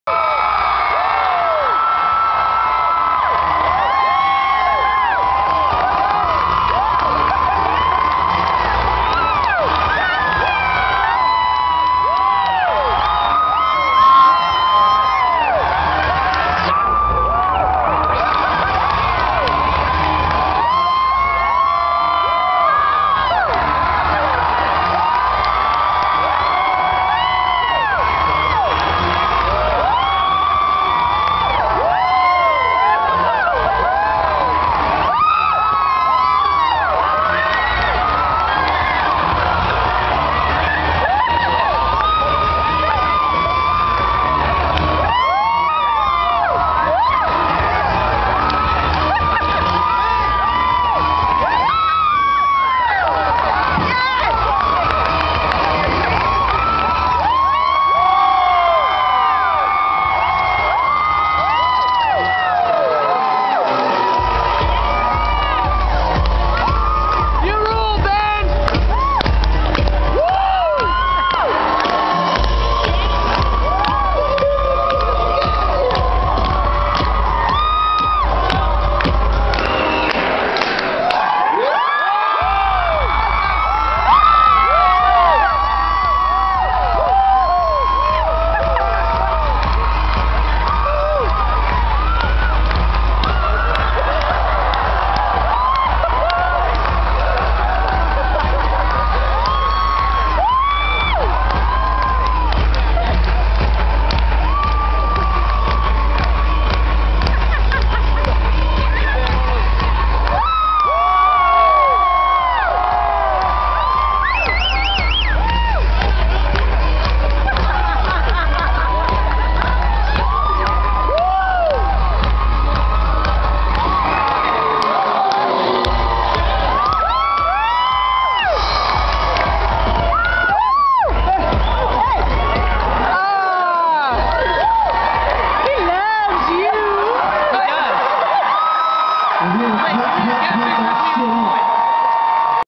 Ben Folds and band going crazy
Ben, Ryan, Sam, Andrew and Chad all running around stage like crazy people. This is how they opened the shows in early 2011. Poor Sam and Ryan have a collision but were just fine!

And featuring my friend Alex Kintzle at the end!